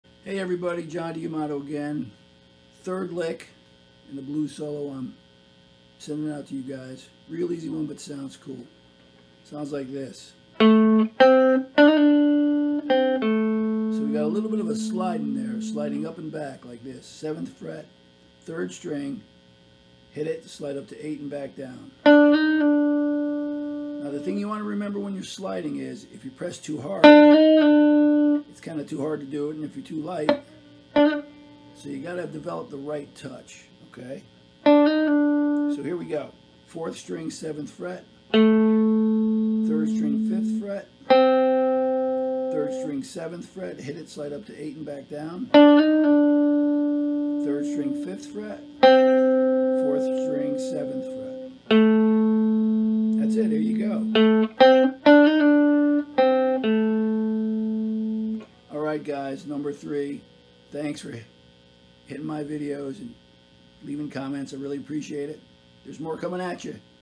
John D'Amato's Easy Blues Solo Phrase 3
Here's the 3rd phrase in the easy blues solo. Put all 3 together. There are more coming soon.
Thanks